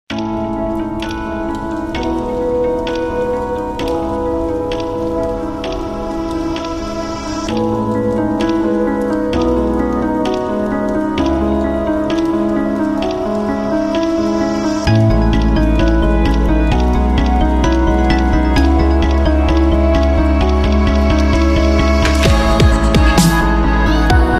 W30cm crystal bowl collection, can use as centerpieces 😍 Creative designs,various colors can choose.
.Popular to use as home decoration, event decoration, and chocolate display.